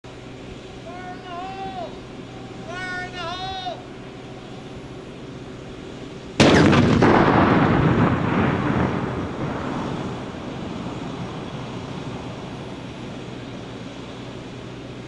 Remote Detonating a Suspicious Honda
Tele-operated CAT 247B using a disruptor on a simulated suspicious vehicle. Remote capability provided by ARA's Modular Robotic Control System.